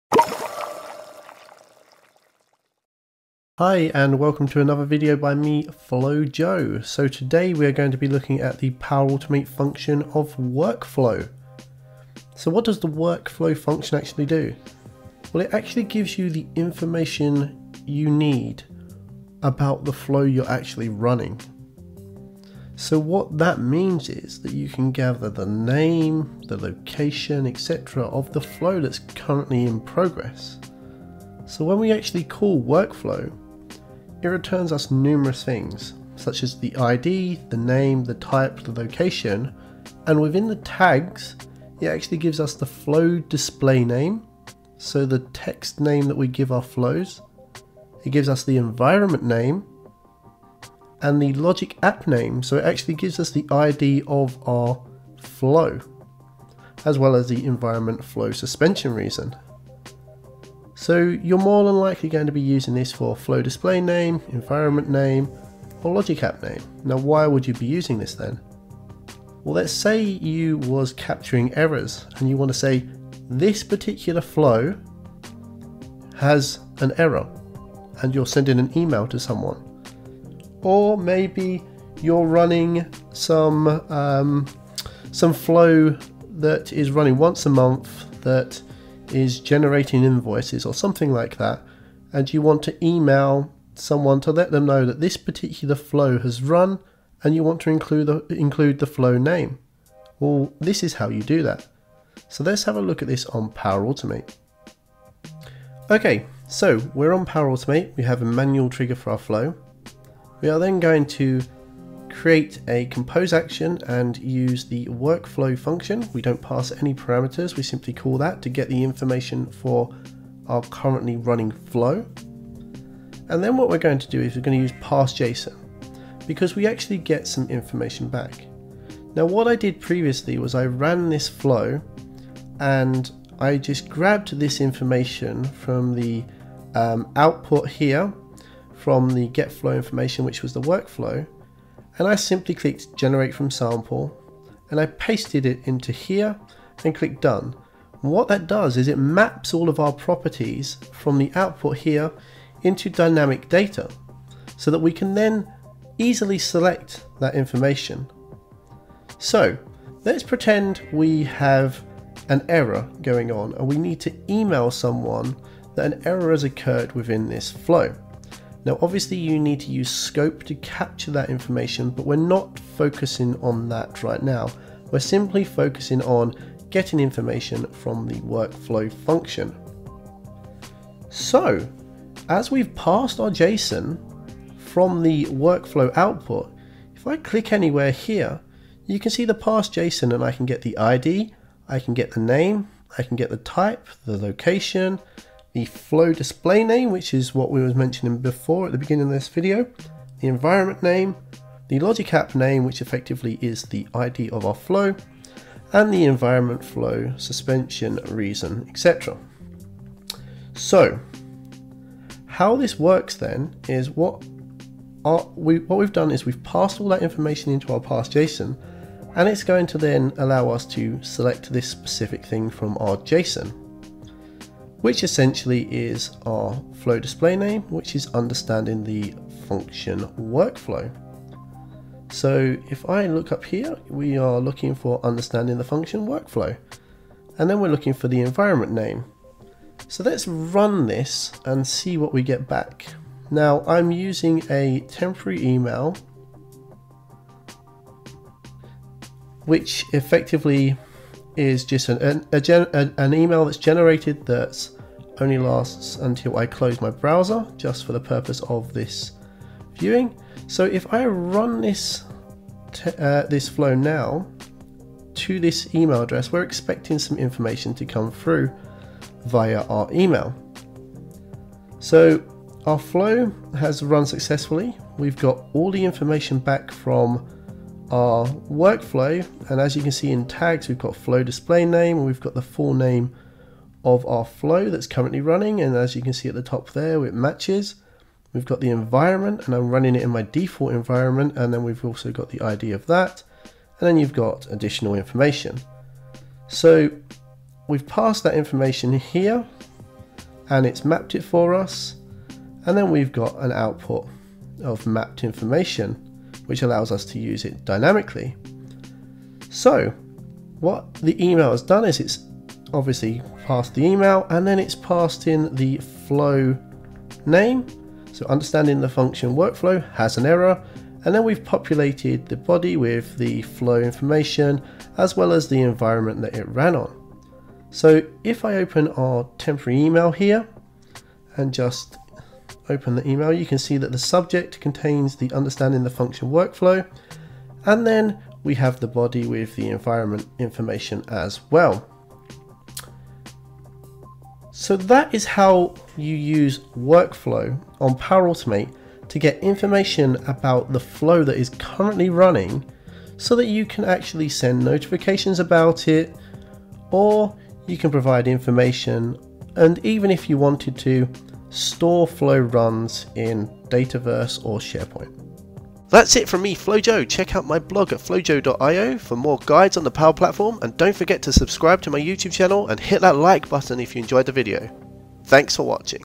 Workflow (Get Information About The Flow Running) Function / Expression | Power Automate
This is a video to show you how to use the workflow function within Power Automate (and Logic Apps!). This function allows you to get information about the Flow that is currently running, including the display name, the environment and more! This video uses an example of emailing the Flow name and environment if an error occurred during the run of the Flow.

Example Function: workflow()